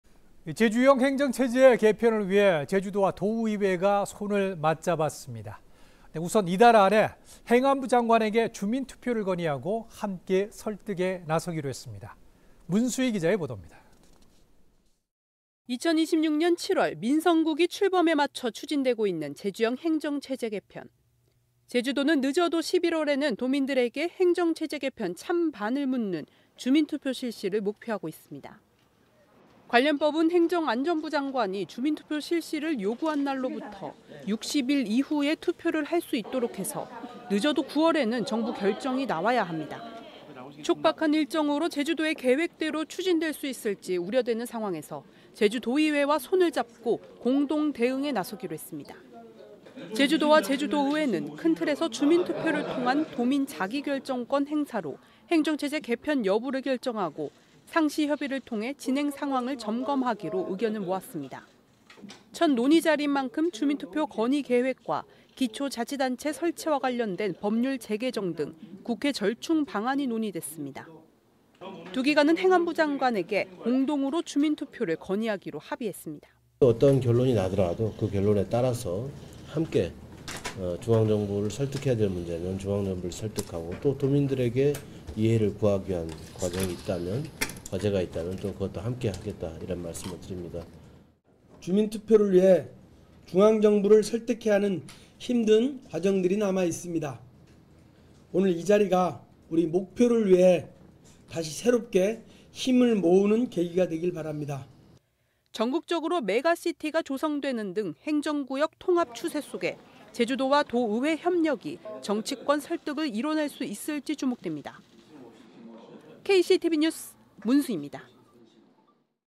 도-의회 행정체제개편 '맞손'…주민투표 공동 건의
제주형 행정체제개편을 위해 제주도와 제주도의회가 손을 맞잡았습니다.

우선 이달 안에 행안부 장관에게 주민투표를 건의하고, 함께 설득에 나서기로 했습니다.

2026년 7월, 민선9기 출범에 맞춰 추진되고 있는 제주형 행정체제개편.

제주도는 늦어도 11월에는 도민들에게 행정체제개편 찬반을 묻는 주민투표 실시를 목표하고 있습니다.

관련법은 행정안전부 장관이 주민투표 실시를 요구한 날로부터 60일 이후에 투표를 할 수 있도록 해서 늦어도 9월에는 정부 결정이 나와야 합니다.

촉박한 일정으로 제주도의 계획대로 추진될 수 있을지 우려되는 상황에서 제주도의회와 손을 잡고 공동 대응에 나서기로 했습니다.

제주도와 제주도의회는 큰 틀에서 주민투표를 통한 도민 자기 결정권 행사로 행정체제 개편 여부를 결정하고 상시 협의를 통해 진행상황을 점검하기로 의견을 모았습니다.

첫 논의 자리인 만큼 주민투표 건의 계획과 기초자치단체 설치와 관련된 법률 제개정 등 국회 절충 방안이 논의됐습니다.

두 기관은 행정안전부 장관에서 공동으로 주민투표를 건의하기로 합의했습니다.

[오영훈 / 제주특별자치도지사]
"어떤 결론이 나더라도 (도의회와) 함께 중앙 정부를 설득할 때는 설득하고 도민들에게 이해를 구할 과정이 있다면 함께 하겠다는 말씀드립니다."

[이상봉 / 제주도의회 의장]
"주민투표를 위해 중앙 정부를 설득해야 하는 힘든 과정이 남아있습니다. 오늘 이 자리가 목표를 위해 다시 새롭게 힘을 모으는 계기가 되길 바랍니다."

전국적으로 메가시티가 조성되는 등 행정구역 통합 추세 속에 제주도와 도의회의 협력이 정치권의 설득을 이뤄낼 수 있을지 주목됩니다.